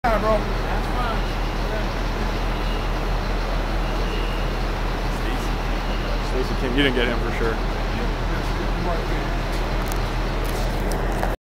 Stacey King signing autographs
stacey was cool and signed 1 each after the game against the twolves